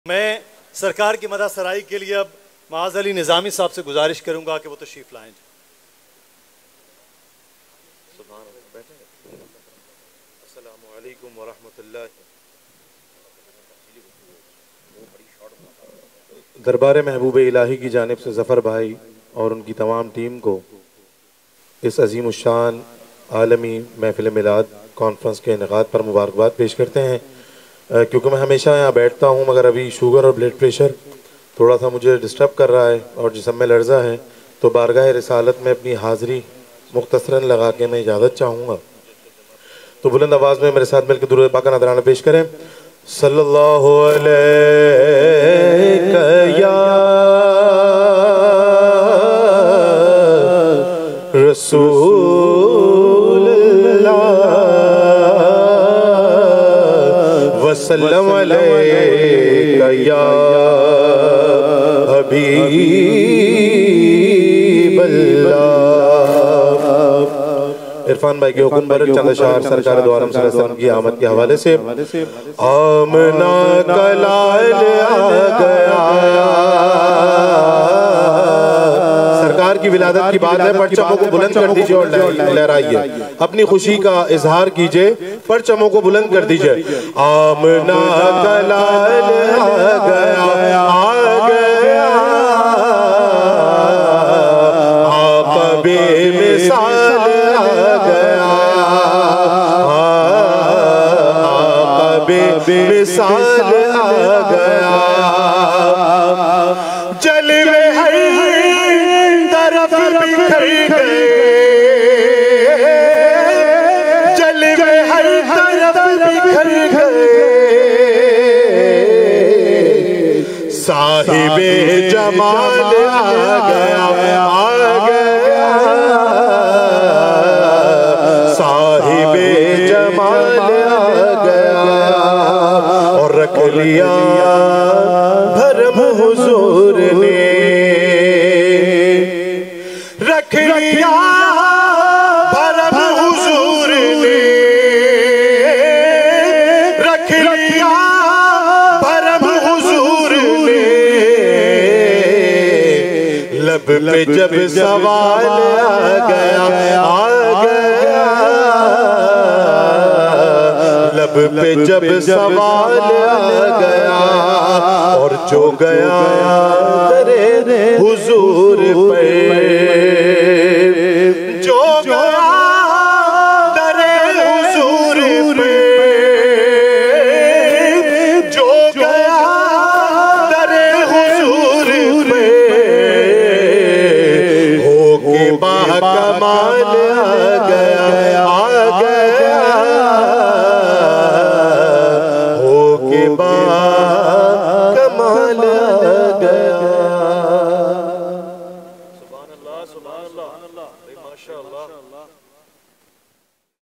Milad e Mustafa (saww) Conference 2023 | Maaz Ali Nizami | Bain ul Mazahib | JDC Organization
بِسْــــــــــــــمِ اللهِ الرَّحْمَنِ الرَّحِيْـــــم

Bain ul Mazahib Milad e Mustafa (saww) Conferance | Maaz Ali Nizami | JDC Organization
جے ڈی سی بین المذاہب قومی میلاد مصطفی ص کانفرنس و چراغاں
JDC Bain-ul-Mazahib Milad-e-Mustafa (SAWW) Confrence 2023

Your Likes Share and Comments will Incourage Us To Make More Benificial Contents For You.